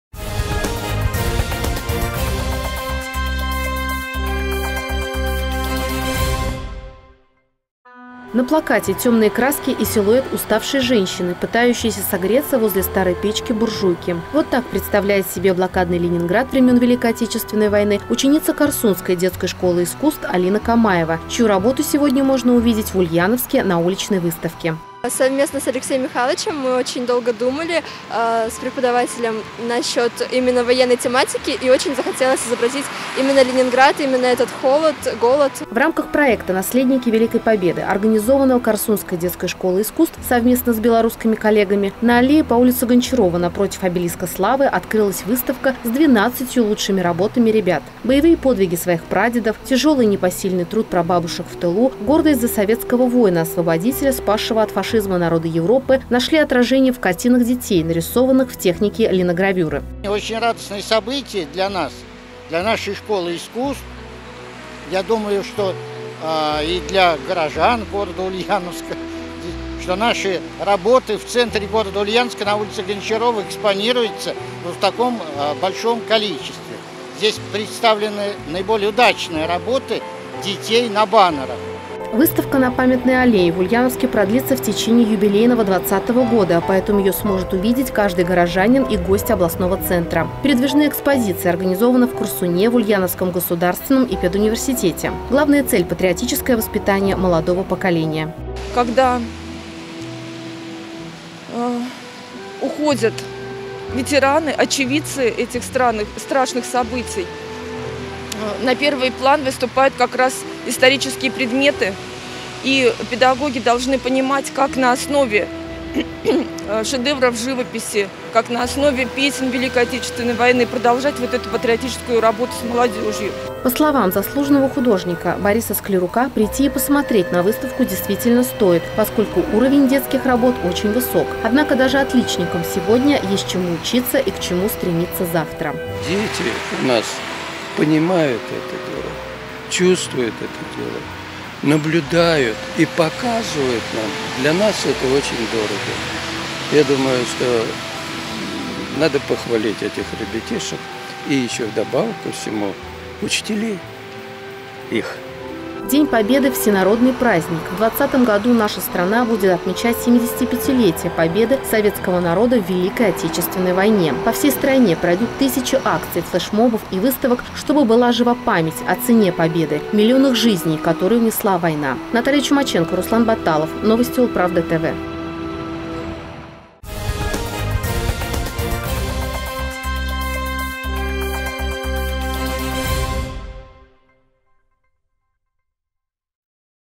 В Ульяновске представили проект «Наследники Великой Победы»
В Ульяновске прошла презентация проекта «Наследники Великой Победы» к 75-летию Победы. Выставка работ с лучшими картинами учеников Карсунской детской школой искусств им. А. Пластова открылась на Аллее по улице Гончарова напротив Обелиска Славы.